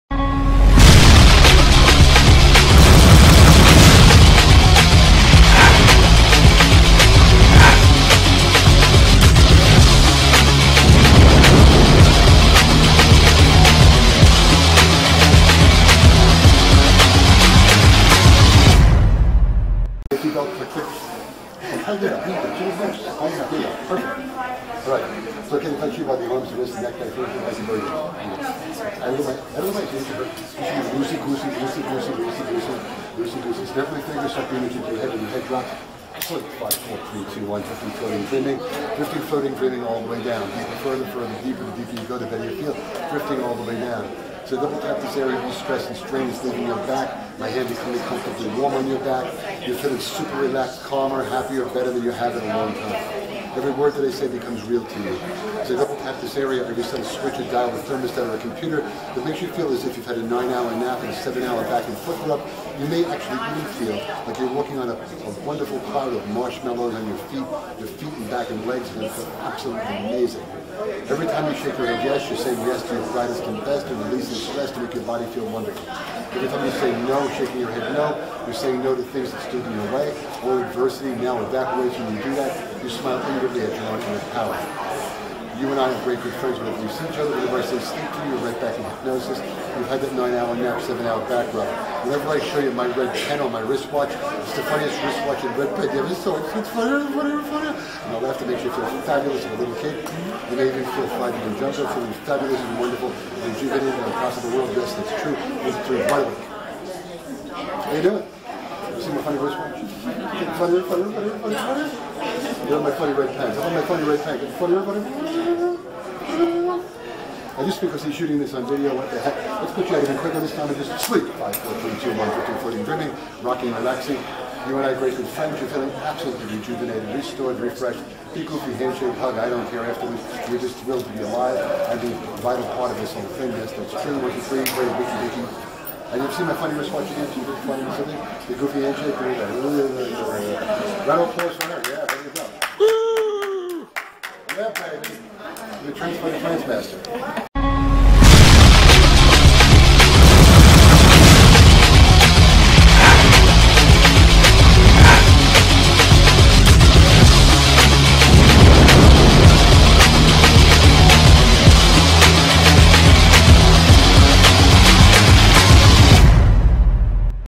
2015 HT Live Clip 2 Stress Relief and Happy - Hypnotist John Cerbone, The Trance-Master
The Trance-Master, Hypnotist John Cerbone hosting Live Speed Trance Hypnosis Demos at HT Live Conference in Las Vegas 8- 2015 - In this clip 2 Stress Relief and Happy, A speed Trance Hypnotic Induction Performed Ice-Cold as you see it here, shot live, and Education Enhancement Suggestions, and relaxation. Fastest Hypnotist Alive!
SpeedTrance.com
HypnosisStageShow.com